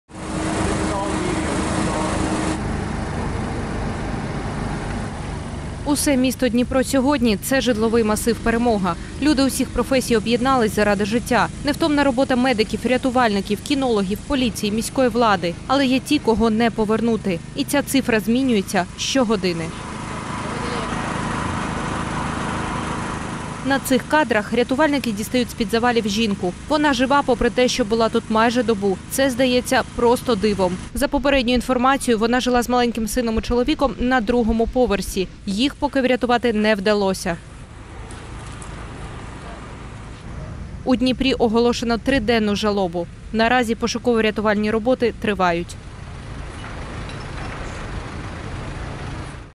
Дніпро – Незламне: місто долає жахливі наслідки влучання російської ракети
У Дніпрі продовжують ліквідувати наслідки терористичного обстрілу росіянами багатоповерхівки в одному з районів міста. Просто серед білого дня в суботу ракета типу х- 22 влучила в будинок, вщент знищивши один під’їзд та майже зруйнувавши сусідні. Подробиці – у сюжеті.